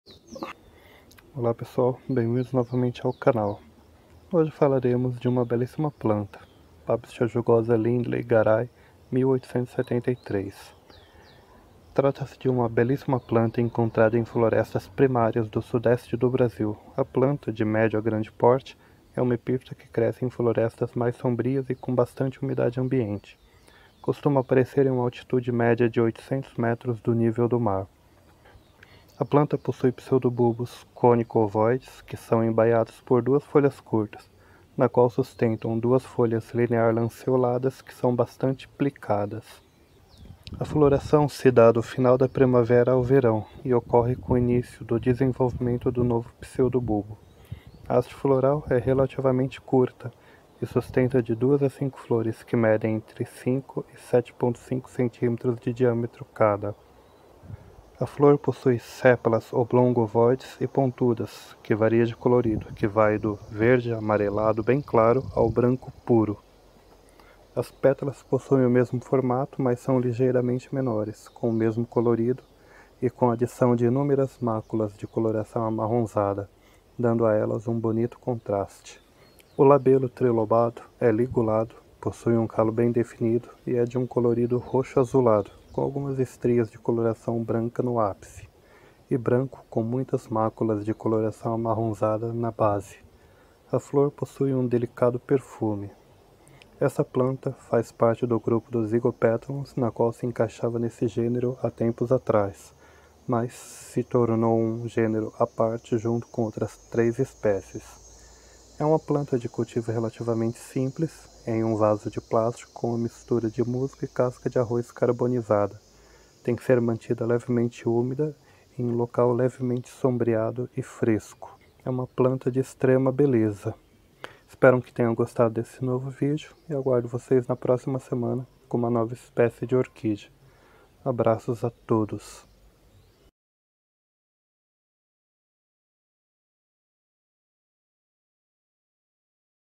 Orquídea; Pabstia jugosa [Lindley] Garay 1873
Olá pessoal, hoje falaremos de uma orquídea que é considerada uma das mais lindas orquídeas nativas do Brasil, que é a Pabstia jugosa [Lindley]Garay 1873. Trata-se de uma belíssima espécie que cresce no sudeste do Brasil, ocorrendo em matas primárias com bastante umidade ambiente e levemente sombrias. A ocorrência desta planta na natureza é um forte indício que a floresta é original, pois dificilmente se avista esta espécie fora deste tipo de habitat. È uma planta que dificilmente passara despercebida e uma exposição, pois ela chama muita a atenção por conta de sua beleza. Ela fazia parte do gênero Zygopetalum, da qual foi retirada anos atrás com outras espécies. Mas mesmo assim carrega características daquele gênero, como por exemplo o fato de suas flores ao serem polinizadas não caírem, e sim permanecerem ainda presas ao ovário da haste floral da planta, adquirindo um colorido esverdeado e assim ajudando na fotossíntese. Espero que apreciem essa belíssima planta.

marcos_aurelio_orquideas

Caso você tenha interesse de conhecer ainda melhor o mundo das Orquídeas, indico este Curso on line com Vídeo Aulas. É só clicar no Link abaixo;